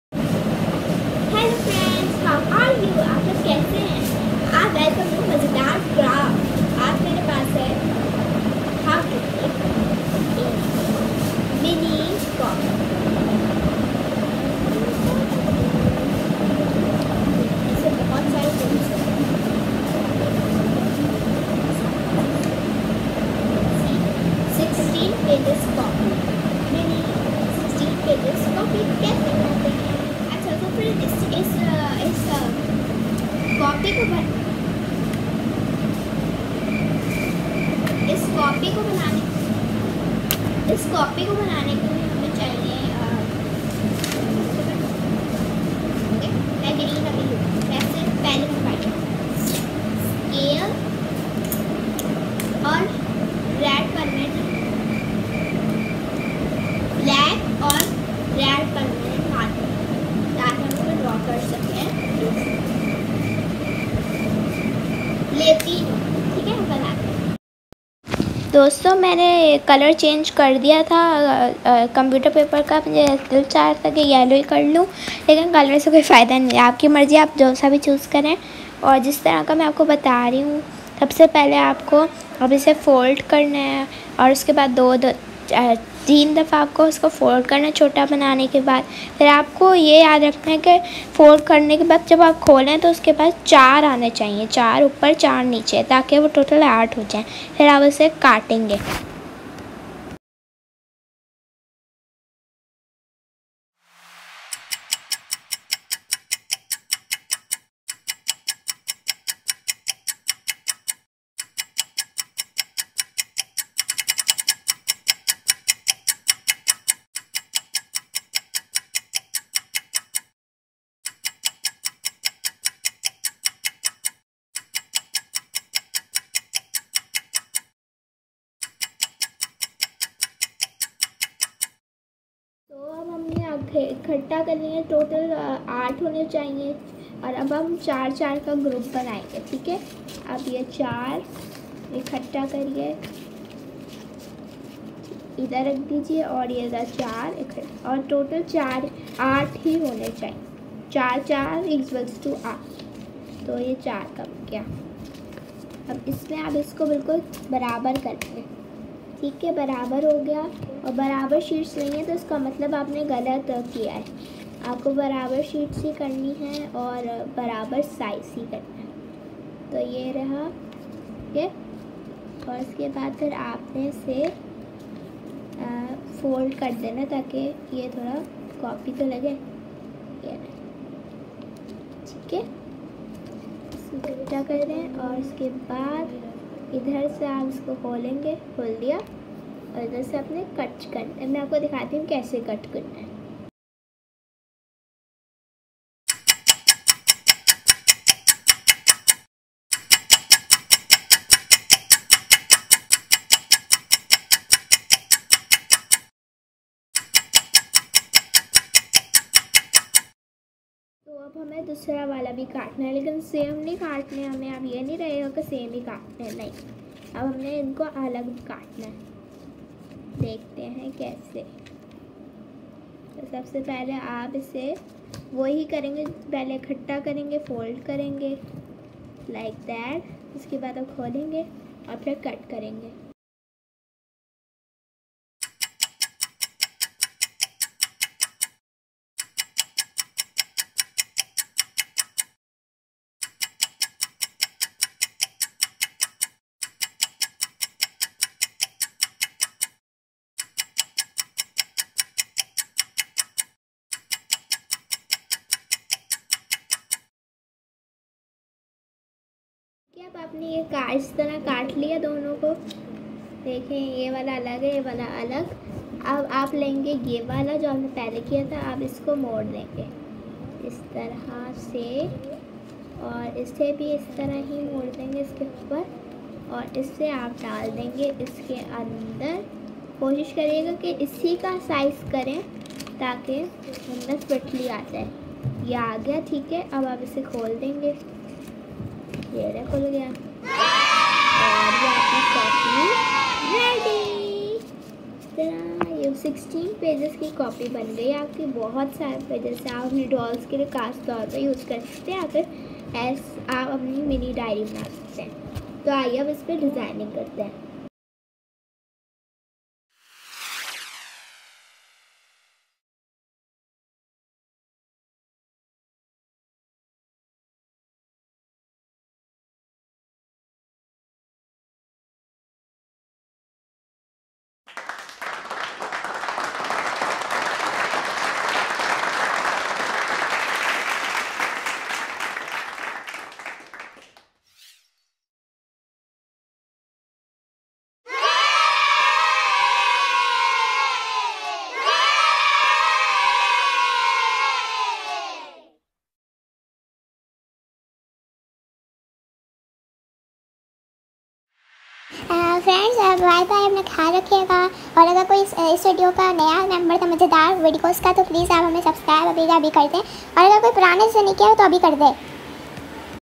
How to make Mini Emoji Copy /Mazaydaar craft
How was this craft?
Watch second part!
Requirements for this craft:
One A4 size computer paper (any color )
Scissors
Black marker
Red marker (for making an emoji)
Scale
Steps:
First a fall take paper
Then fold it as you fold it for making card
Then fold it two times to make small (as shown in video )
After it open it and it should be eight portions
Then cut them with scale
Then make four pages group
Take one and fold it
Take another and then fold it
Then again take first one and cut it as shown in video
Take second and cut it as shown in video
then join the first group with second
And it's ready.
Make an emoji of your choice